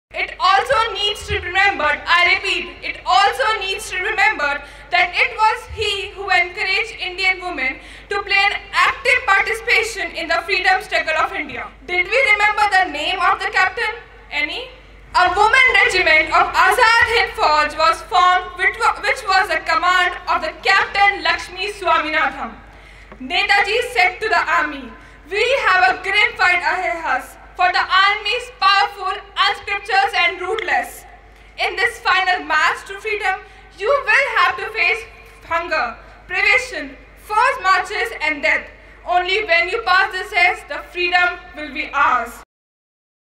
The first women Capt. of Indian National Army - Capt. Lakshmi Swaminathan
Did you know that the Indian National Army had its first woman as a Captain way back in the 1940s?
Watch a student of Subharti University - Ms. Chitrangi Bhardwaj remind us of Lakshmi Swaminathan who was a revolutionary of the Indian independence movement, an officer of the Indian National Army, and the Minister of Women's Affairs in the Azad Hind government.